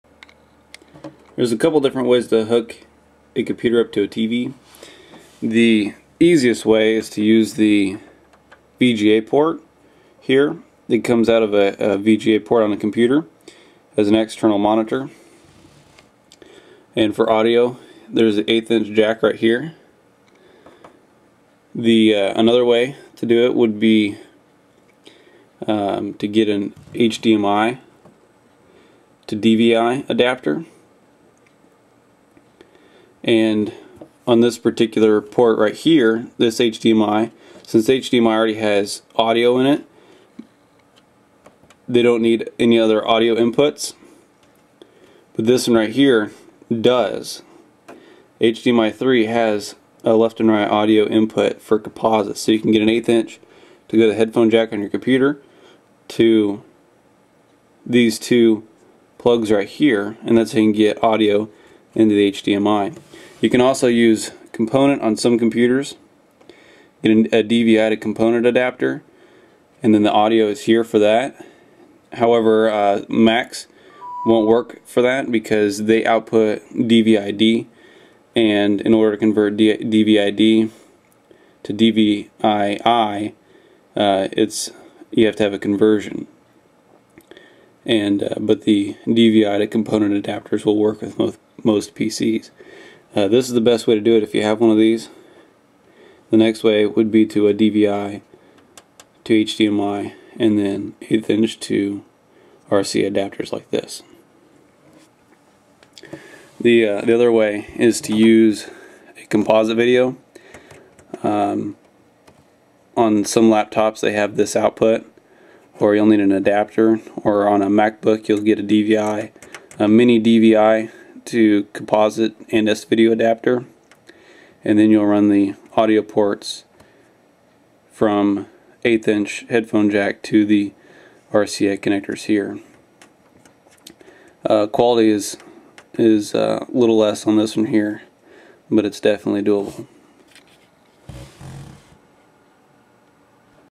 Hooking up computer to Flat screen tv
Was made to better describe attaching a computer to TV